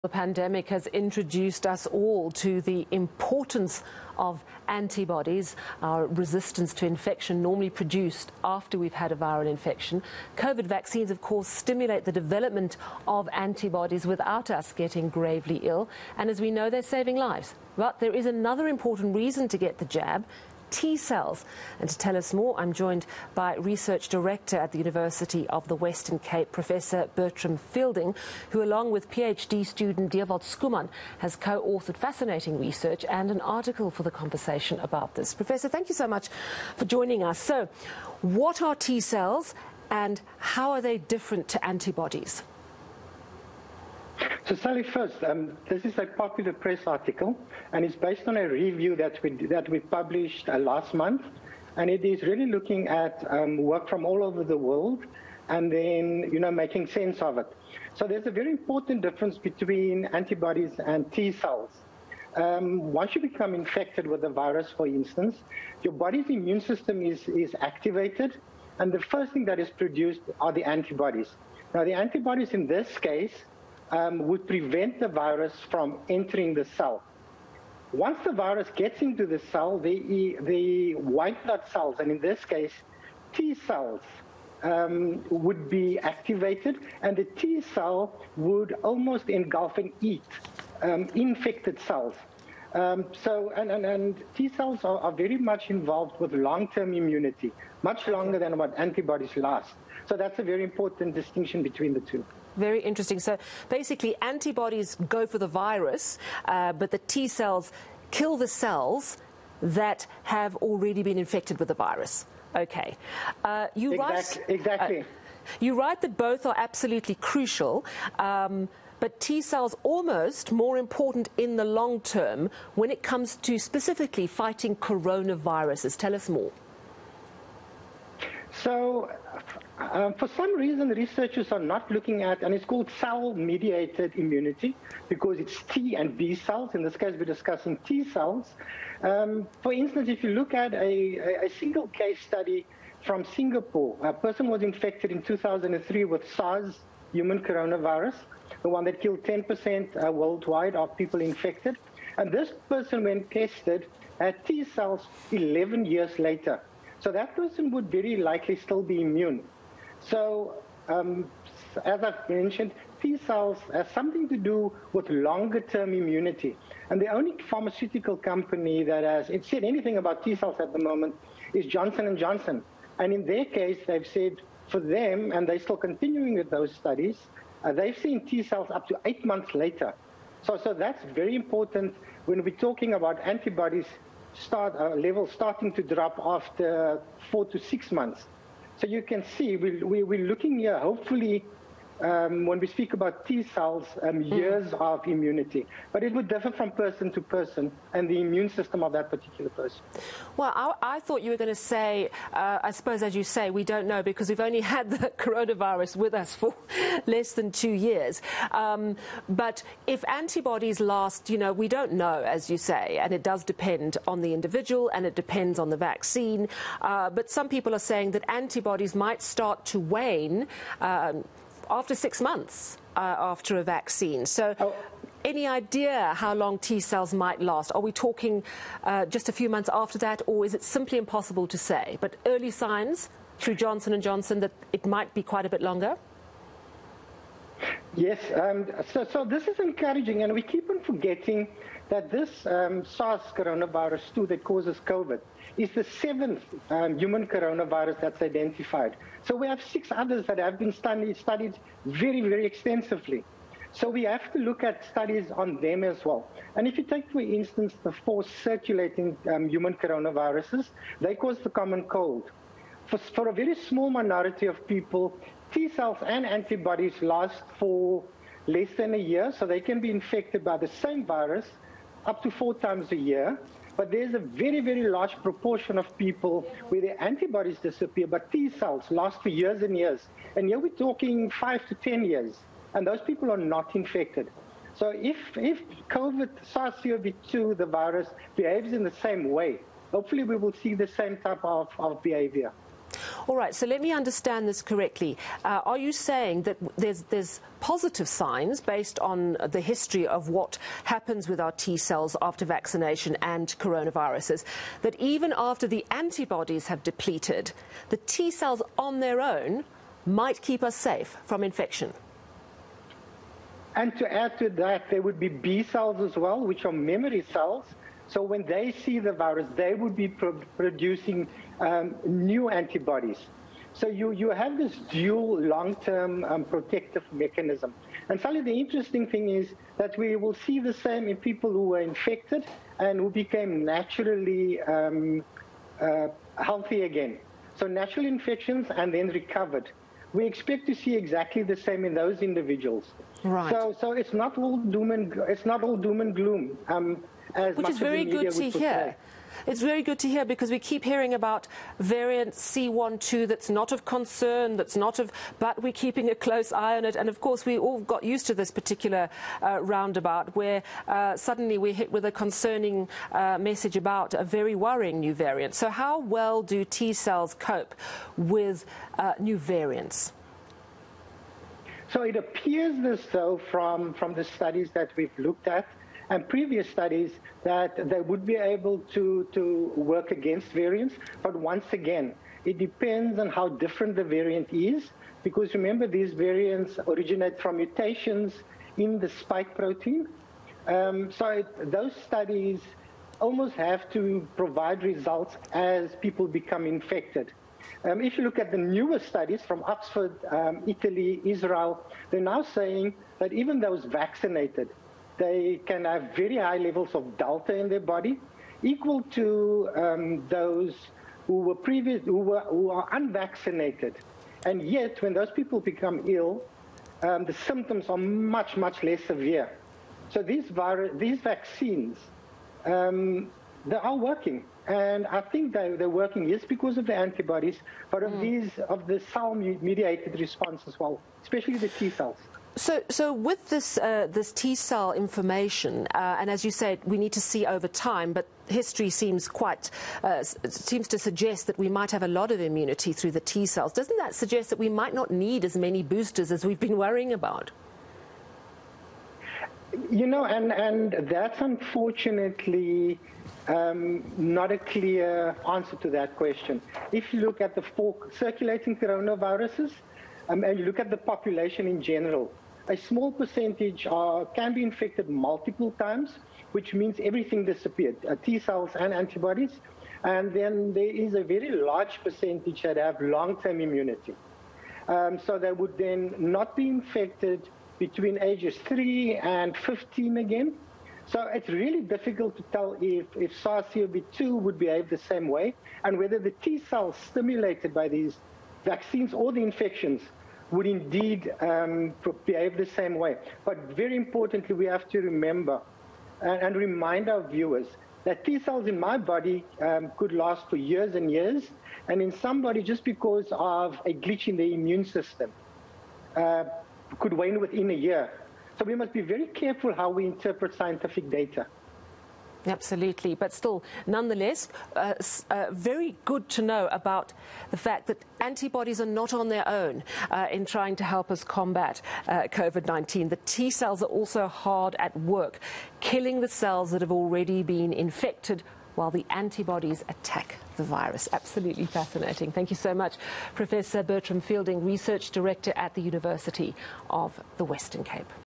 COVID-19 | The Importance of antibodies and T Cells
The pandemic has introduced us all to the importance of antibodies – our resistance to infection, normally produced after we have had a viral infection. Burtram Fielding, who along with PHD student Dewald Schoeman, has co-authored fascinating research and an article for The Conversation about this speaks to eNCA Courtesy